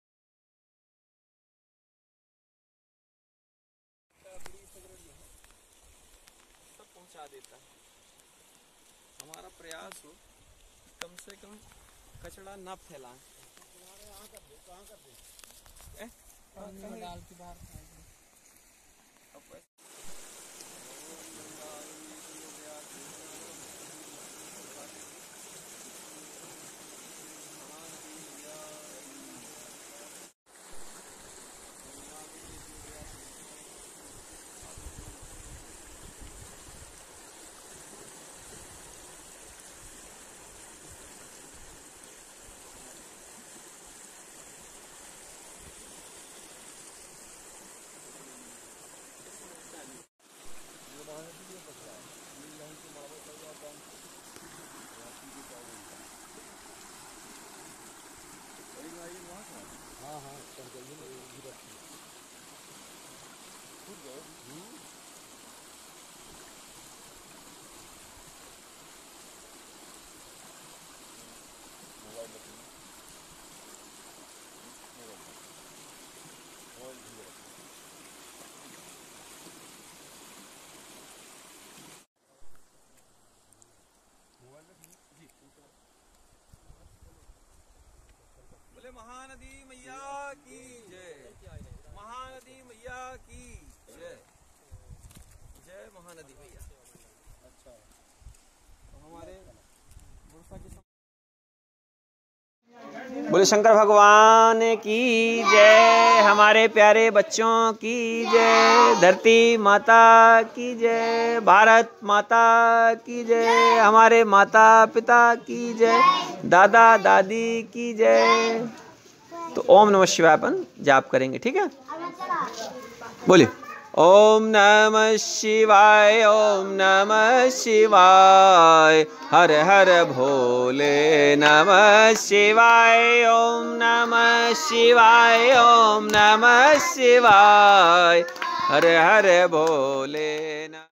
भुङसा संगम घाट जिला कटनी।महानदी मैया की गोद में सफ़ाई पूजन।कर्मयोगी प्रकृति पुत्र त्यागीजी7566239315
*भुङसा माध्यमिक विद्यालय में आध्यात्मिक ऊर्जा। जन्म भूमि के प्रति जगे प्रेम*
राष्ट्रभक्त संत कर्मयोगी प्रकृति पुत्र त्यागी जी आश्रम विश्व सेवा समिति ज्ञान तीर्थ स्वर्ग धाम विलायत कला कटनी मध्य प्रदेश भारत जहां होता है संस्कृति का उत्थान मानव बने देवता